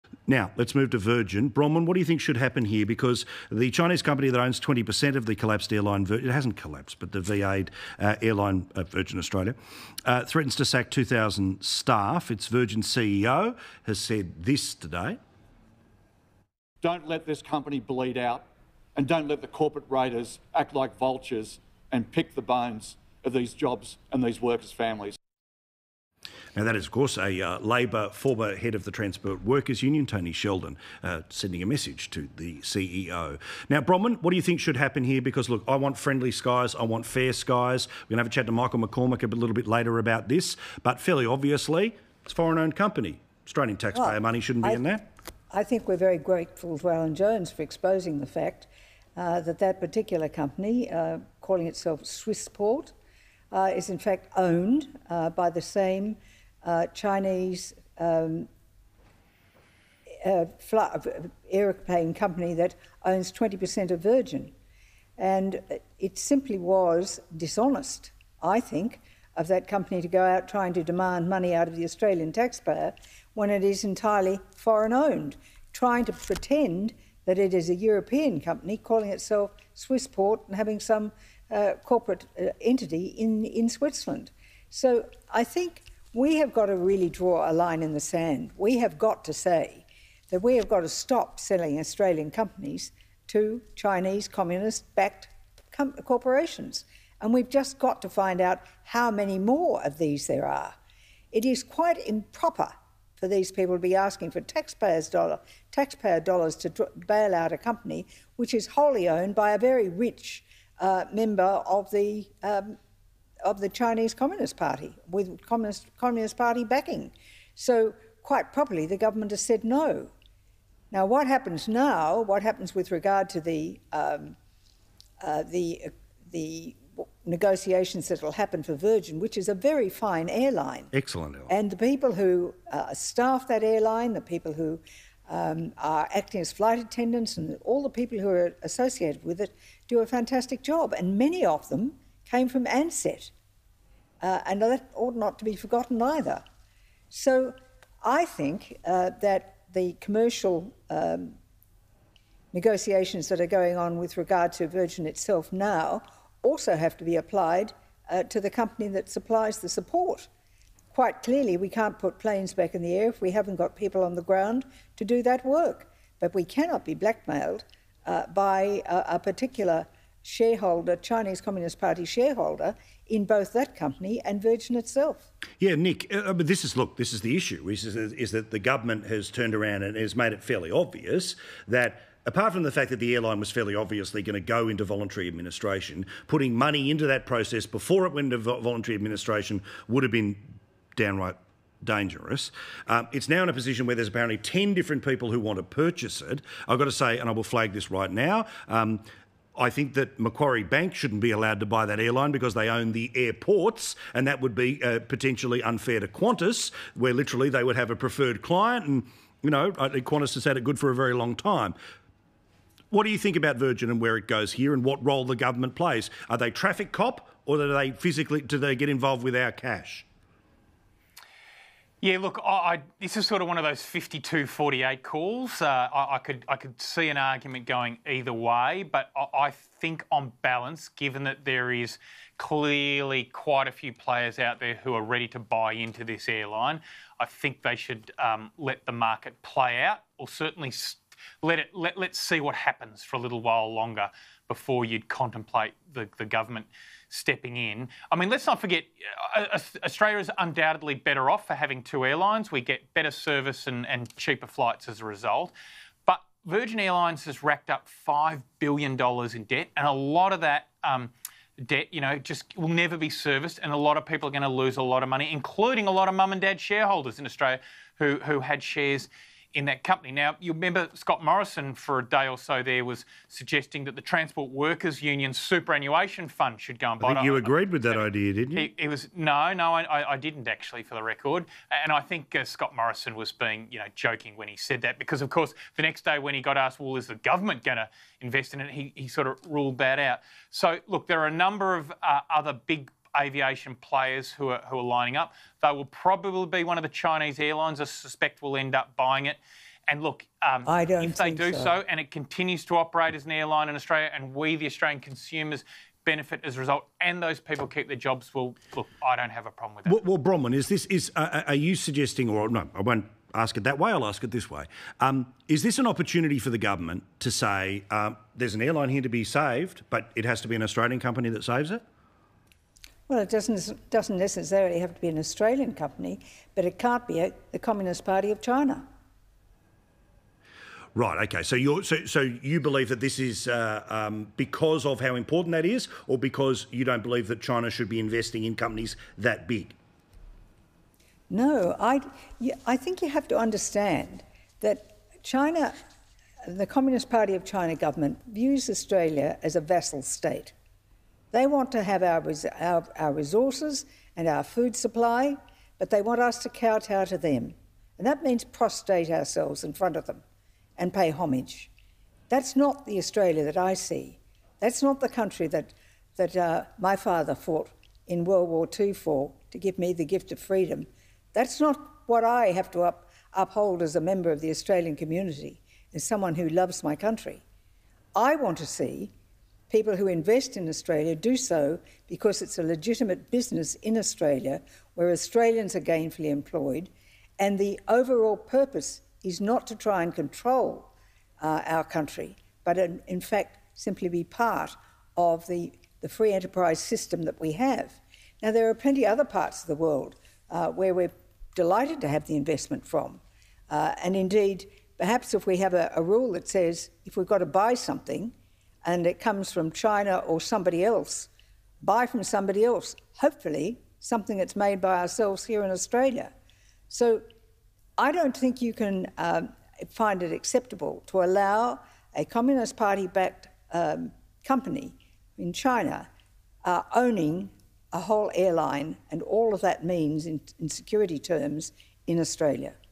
'Communist Party of China views Australia as a vassal state': Bronwyn Bishop
Former speaker of the house Bronwyn Bishop says as Virgin Australia struggles to keep afloat, "we have got to stop selling Australian companies to Chinese communist backed corporations".

"It's quite improper for these people to be asking for taxpayer dollars to bail out a company which is wholly owned by a very rich member of the Chinese communist party, with communist party backing".

Ms Bishop was referring to Swiss Port, a major supplier for Australian ground control facilities and personnel, which along with Virgin requested a government bailout.

"We cannot be blackmailed by a particular Chinese communist party shareholder," she said.

Ms Bishop told Sky News host Paul Murray, it doesn't have to be an Australian company who takes over Virgin, "but it can't be a communist party of China" owned or backed corporation.

"The Communist Party of China government views Australia as a vassal state".

She said China wants Australia for "our resources and our food supply, but they want us to kowtow to them".

Image: AP